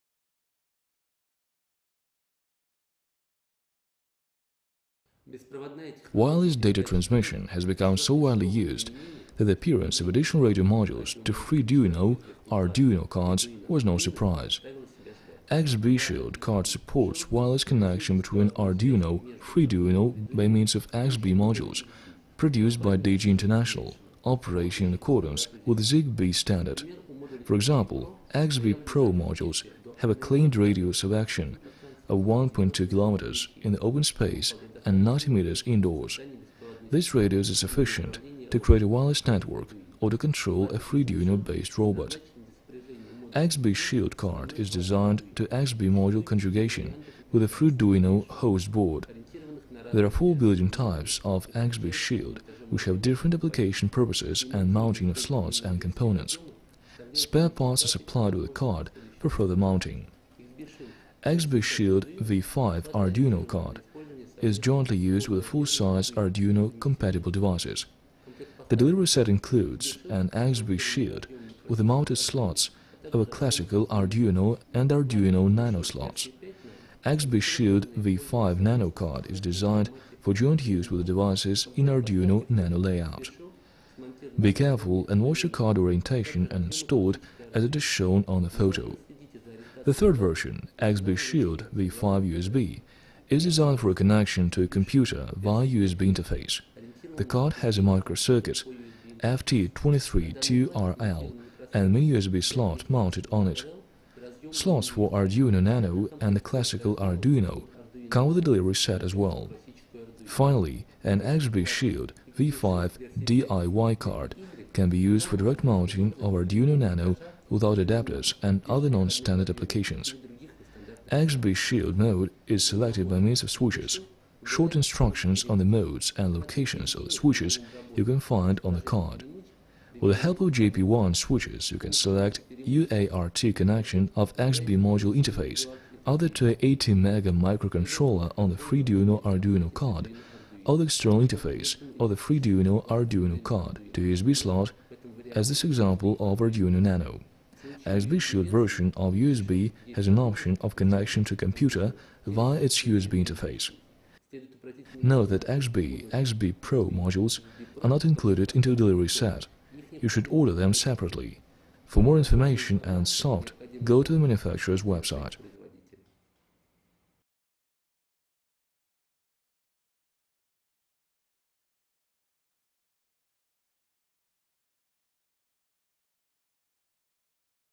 XBee Shield, a card for wireless modules XBee ...
XBee Shield, a card for wireless modules XBee connectionWireless data transmission has become so widely used that the appearance of additional radio modules to Freeduino/Arduino cards was no surprise.XBee Shield card supports wireless connection between Arduino/Freeduino by means of XBee modules produced by Digi International operating in accordance with the ZigBee standard. For example, XBee-PRO modules have a claimed radius of action of 1.2 km in the open space and 90 m indoors. This radius is sufficient to create a wireless network or to control a Freeduino-based robot.XBee Shield card is designed to XBee module conjugation with the Freeduino host board. There are 4 bundling types of XBee Shield which have different application purposes and mounting of slots and components. Spare parts are supplied with the card for further mounting.XBee Shield v5 Arduino card is jointly used with full-size Arduino-compatible devices. The delivery set includes an XBee Shield with mounted slots of a "classical" Arduino and Arduino Nano slots. XBee Shield v5 Nano card is designed for joint use with devices in the Arduino Nano layout.Be careful and watch the card orientation and install it as it is shown on the photo.The third version - XBee Shield v5 USB - is designed for a connection to a computer via USB interface. The card has a microcircuit FT232RL and mini-USB slot mounted on it. Slots for Arduino Nano and a "classical" Arduino come with the delivery set as well.Finally, an XBee Shield v5 DIY card can be useful for a direct mounting of Arduino Nano without adapters and other non-standard applications. XBee Shield mode is selected by means of switches. Short instructions on the modes and locations of the switches you can find on the card. With the help of JP1 switches you can select UART connection of XBee module interface either to a ATmega microcontroleer on the Freeduino/Arduino card, or to the external interface of the Freduino/Arduino card (to a USB slot -- as this example of Arduino Nano). XBee Shield version of USB has an option of connection to a computer via its USB interface.Note that XBee/XBee-PRO modules are not included in the delivery set. You should order them separately. For more information and soft go to the manufacturer's site.